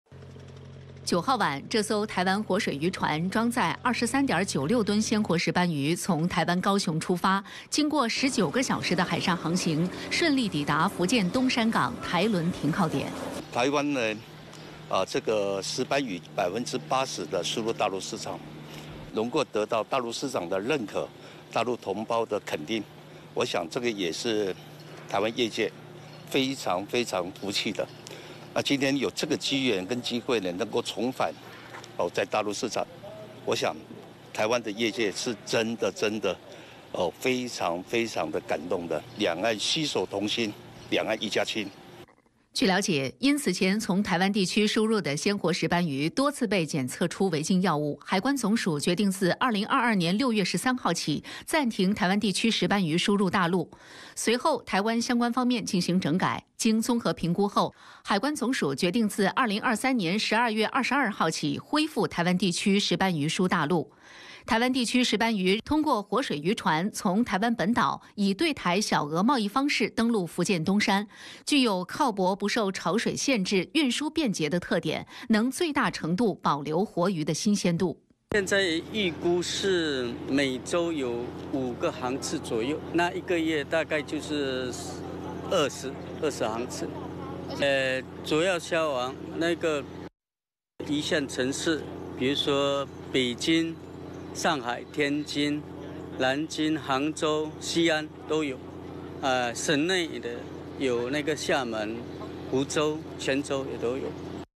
【兩岸】兩岸鮮活水產品航線復航 首批台灣石斑魚運抵福建
23.96噸台灣地區石斑魚日前運抵福建東山港台輪停靠點，這標誌著兩岸鮮活水產品航線正式復航。